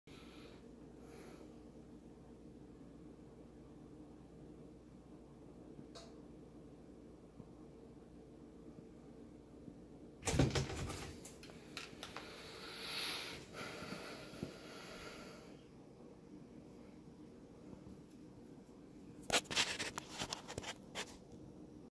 Retic Feeding.
This is Geras, A Goldenchild Tiger Reticulated Python eating a frozen/thawed Jumbo Rat.